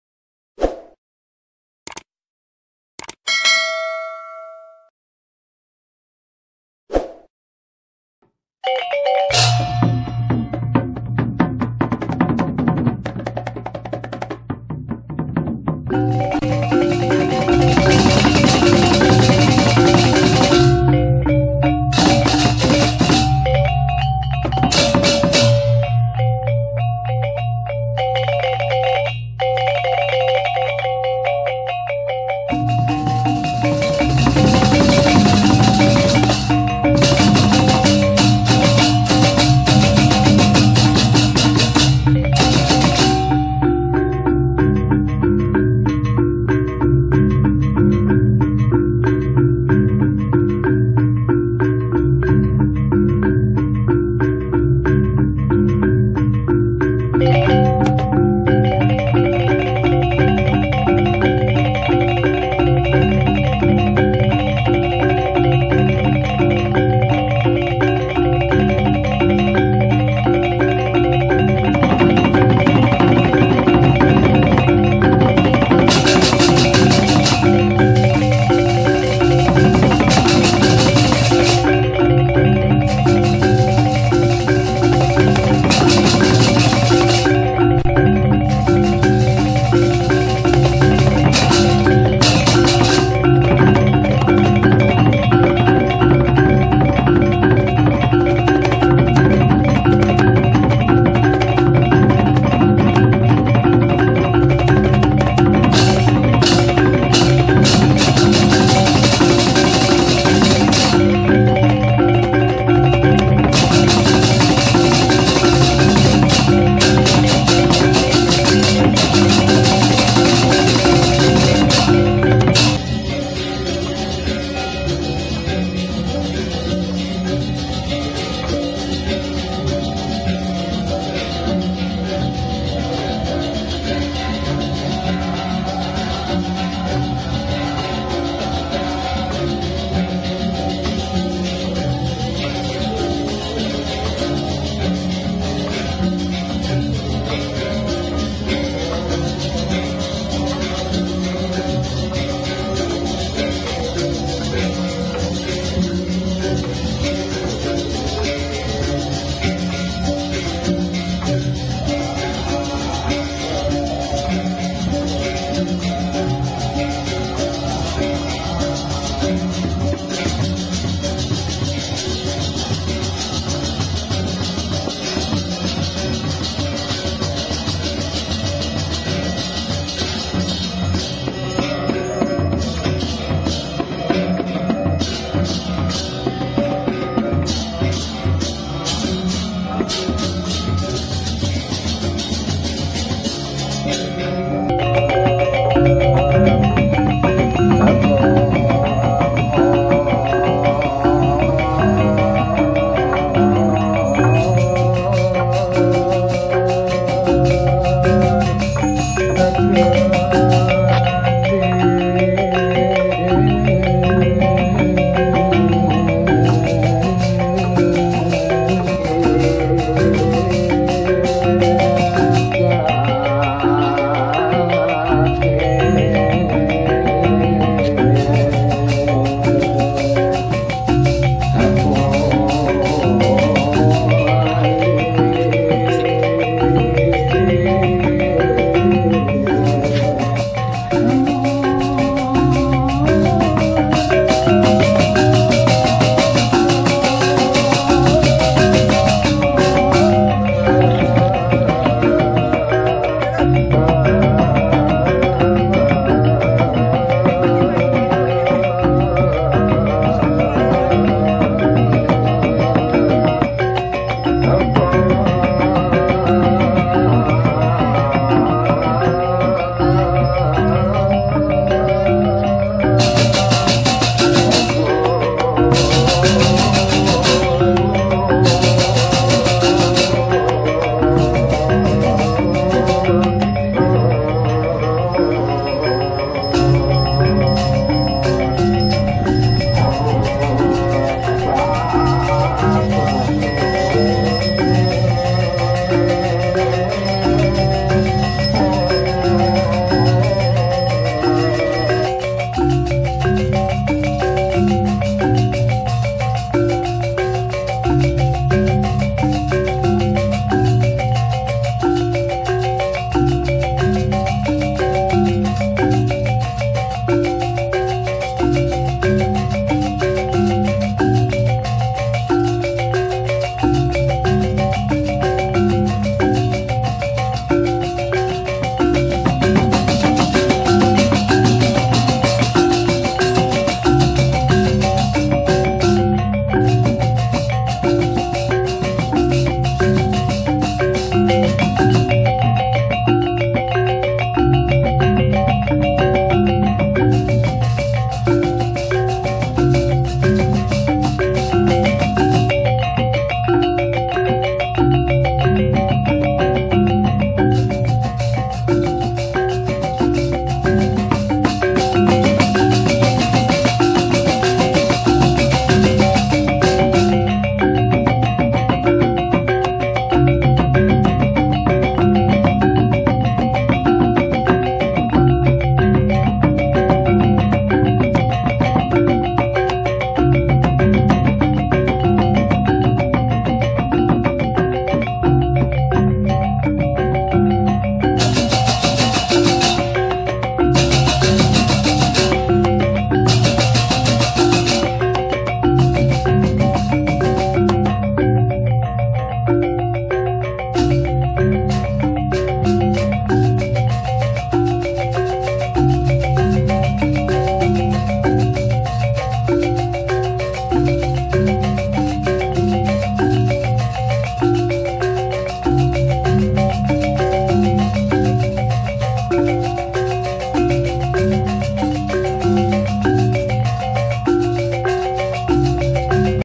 piodalan rahina tilem ring pura dalem desa adat juwuk manis
sedikit video piodalan ring pura dalem desa adat juwuk manis kecamatan pekutatan kabupaten jembrana bali